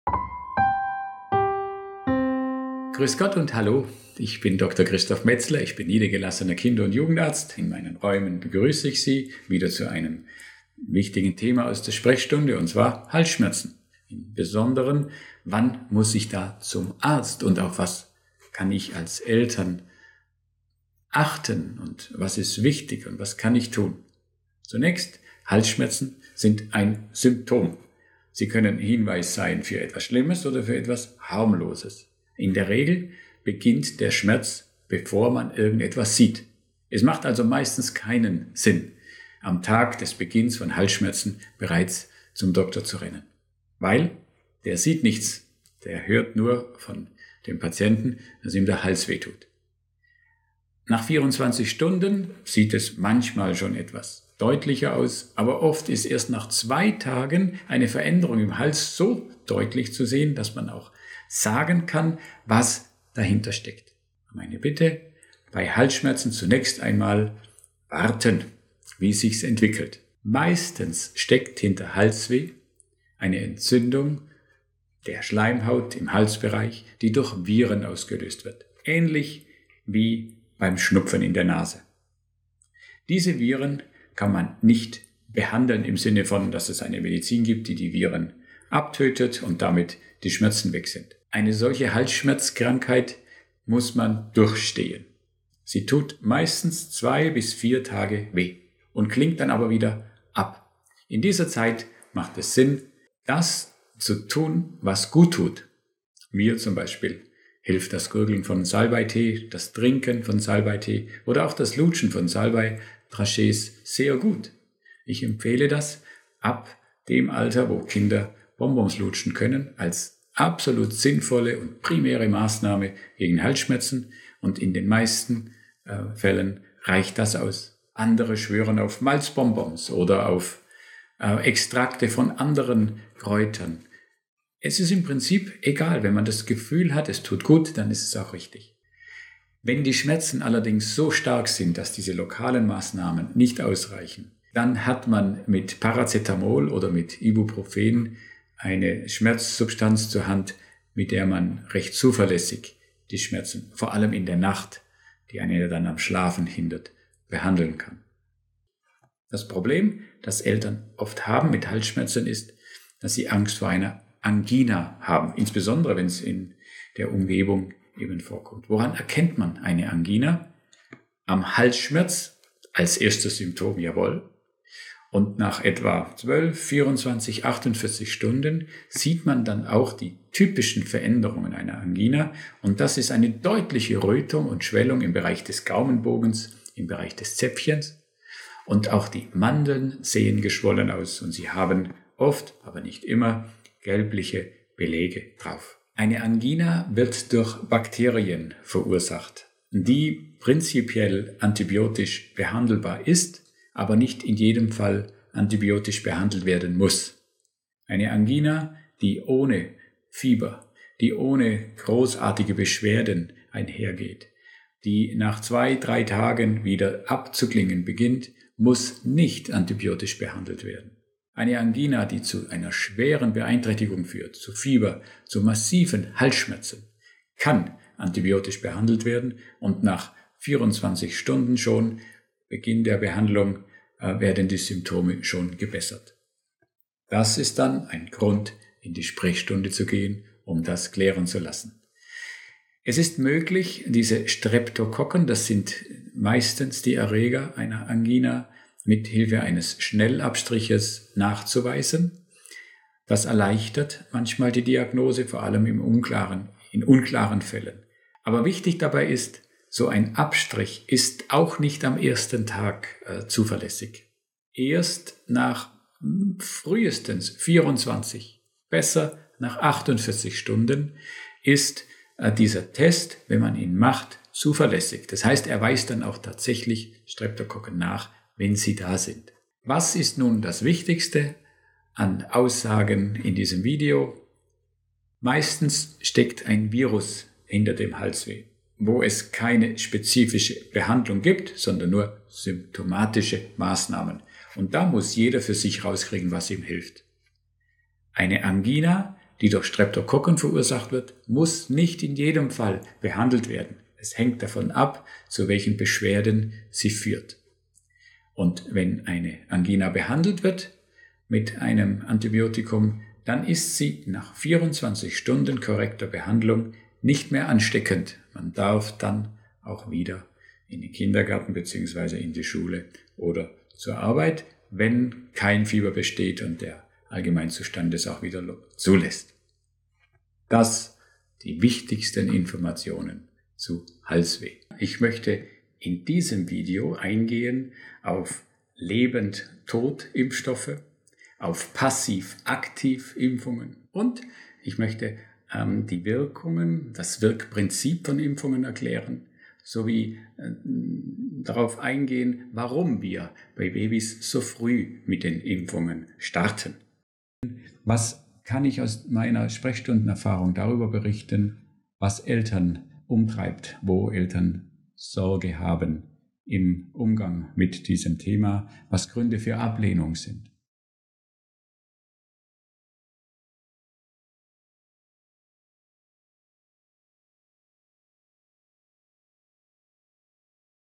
Halsschmerzen - Der Kinderarzt vom Bodensee
Aus seiner persönlichen und seiner Sprechstundenerfahrung heraus spricht der Kinder- und Jugendarzt Dr. med. Christof Metzler über Halsschmerzen. Er geht auf mögliche Ursachen ein und beschreibt was Sie tun können um en Schmerz zu lindern.